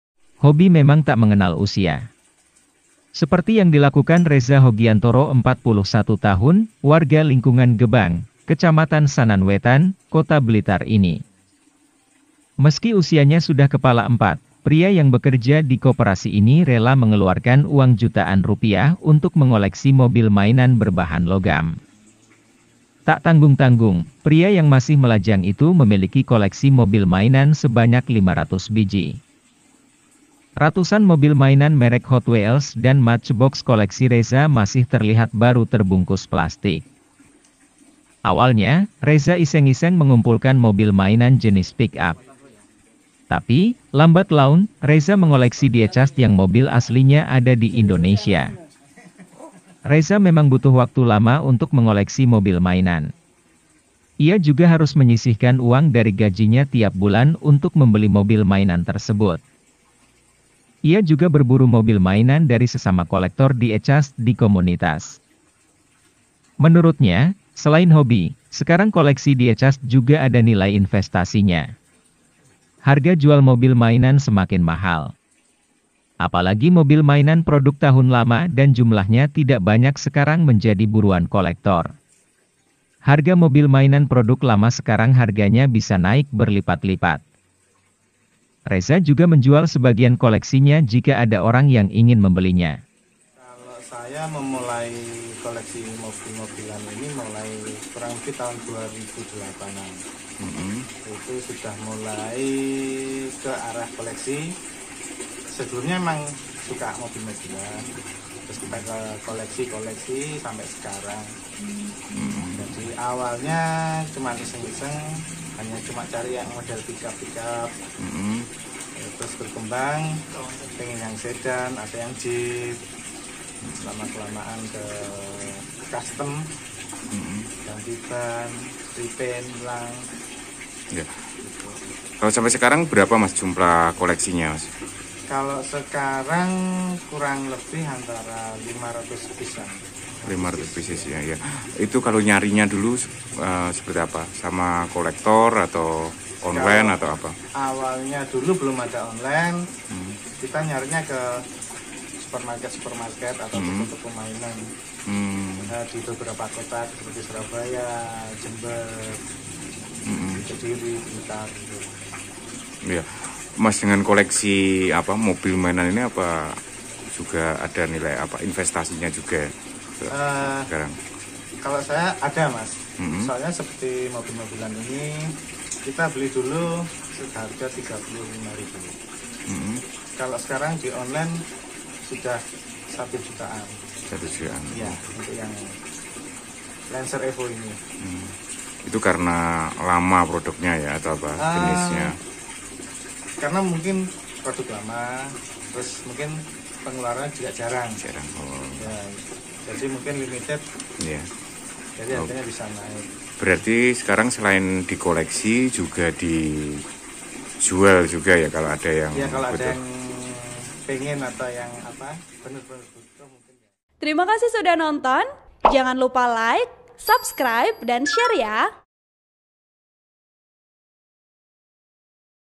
Pria di Kota Blitar Rela Rogoh Uang Jutaan Rupiah untuk Koleksi Mobil Mainan
VP: AGUS

TRIBUNJATIM, BLITAR - Hobi memang tak mengenal usia. Seperti yang dilakukan, Reza Hogiantoro (41), warga Lingkungan Gebang, Kelurahan/Kecamatan Sananwetan, Kota Blitar, ini.

Penulis: Samsul Hadi
Editor: eben haezer